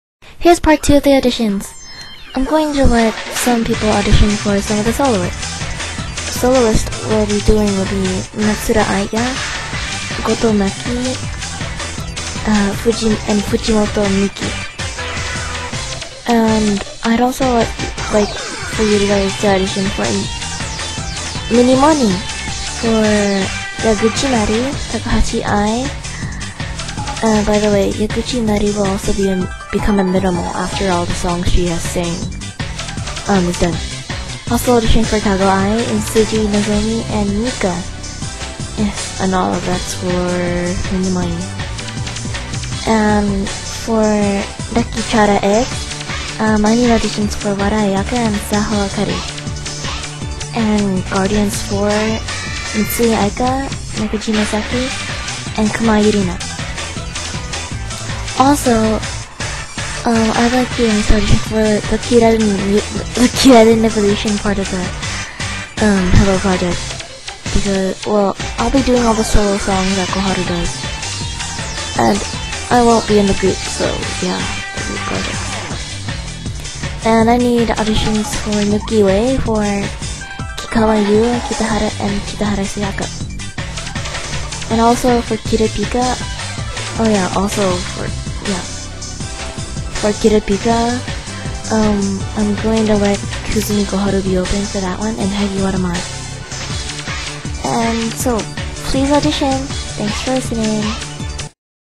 (CLOSED!) EGGS/MINIMALS STILL NEEDED Rakii! Project Part 2 Auditions!
Heres part 2 of the audition!!x33 Remember, MUST BE ACTIVE OR ELSE YOU WILL BE GRADUATED!!!
Also audition for the minimals that will be doing the misc of Hello! Project.
Fill out:
Stage Name:
Voice-Type:
Groups Your Auditioning for:
Who your auditioning for:
Can you mix?:
Will you be active?:

Rakii Way:
Koharu Kusumi: Yumi(SugoiiYumiChan)
Kikkawa Yuu:Miya(TokyoStarx)
Kitahara Sayaka:Lilykoy(therealallisons)

Kira* Pika:
Kusumi Koharu:Seaira-chan(lilspiritgurl)
Hagiwara Mai:Mokachii(animeobsessed4 0)

Soloists:
Goto Maki:
Matsuura Aya:Lilykoy(therealallisons)
Fujimoto Miki:
Kusumi Koharu: Yumi(SugoiiYumiChan

Rakii Chara Eggs!
Wada Ayaka:Lilykoy(therealallisons)
Maeda Yuuka: Yumi (SugoiiYumiChan)
Fukuda Kanon:Queenie(xQueenMusumex)
Saho Akari:Miya(TokyoStarx)

Guardians 4
Mitsui Aika:Miya(TokyoStarx)
Nakajima Saki:Mokachii(animeobsessed4 0)
Kumai Yurina:Chisato(BerryzLoveProject)
Sugaya Risako:Queenie(xQueenMusumex)

Mini Moni:
Yaguchi Mari:Diana(lily14895)
Kago Ai:Mokachii(animeobsessed40)
Tsuji Nozomi:Yumi(SugoiiYumiChan)
Mika:Chi-chan(TheMCGaki)
Takahashi Ai:Sakaza(SatomiKazama)